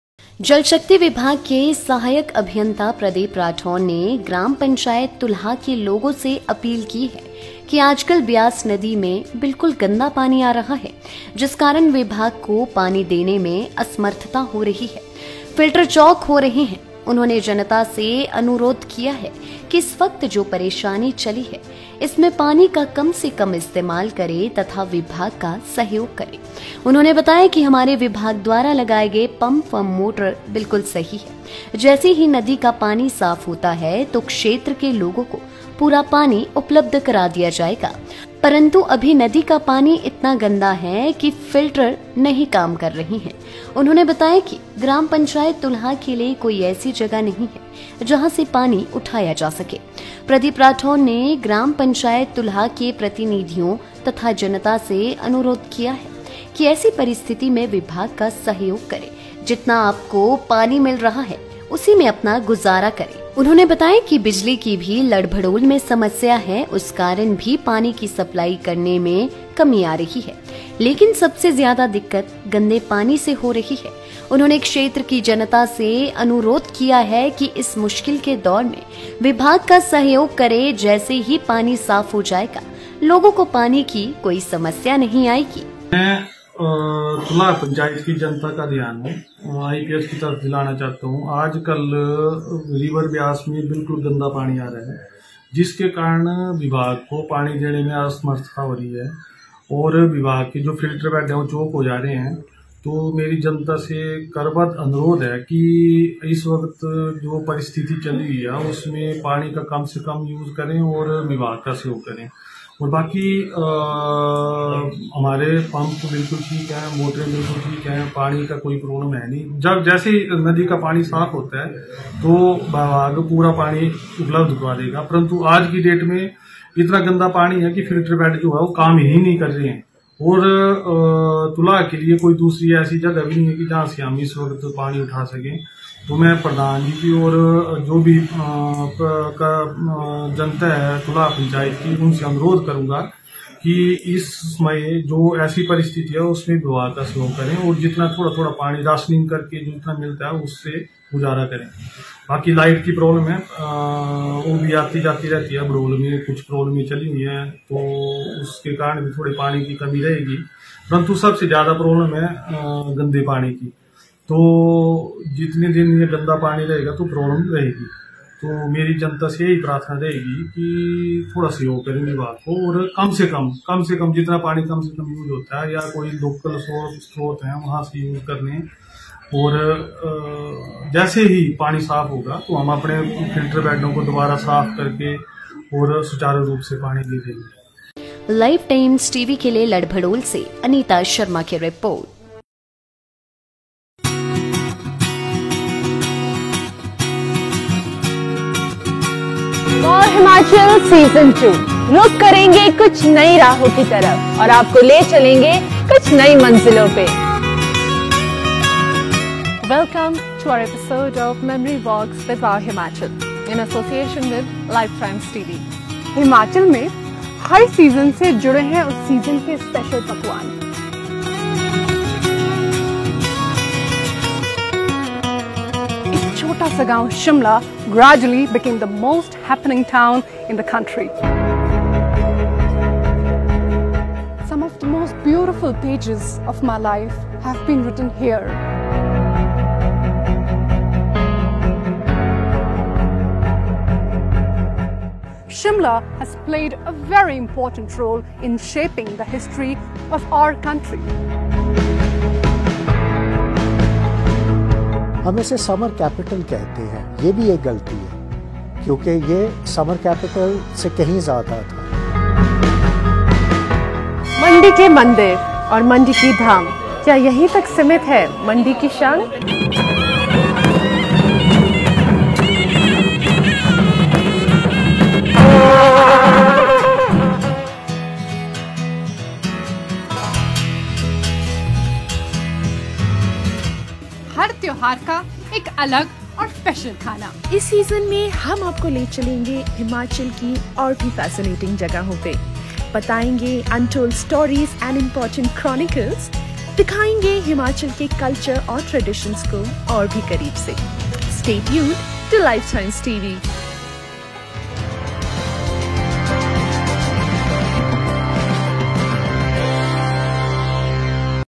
लड़भडोल : व्यास नदी में आ रहा गंदा पानी, लोग करें सहयोग : प्रदीप राठौर
Ladbhadol: Dirty water coming in Vyas river, people should cooperate: Pradeep Rathore